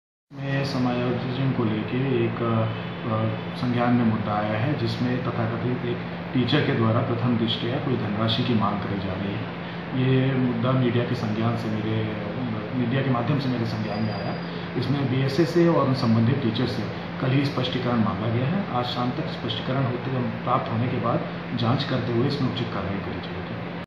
BSA का खास टीचर मनचाहा स्कूल दिलाने के ले रहा था 50 हजार, स्क्रीनशॉट वायरल
हरदोई: शिक्षक भर्ती प्रक्रिया में मनचाहे स्कूल दिलाने के लिए पैसे की मांग करने का मामला सामने आया है. बीएसए के एक खास शिक्षक का व्हाट्सएप चैटिंग का स्क्रीनशॉट वायरल हुआ है. जिसमें वह विद्यालय दिलाने के नाम पर सौदेबाजी करता नजर आ रहा है. डीएम ने जांच के आदेश दिए हैं.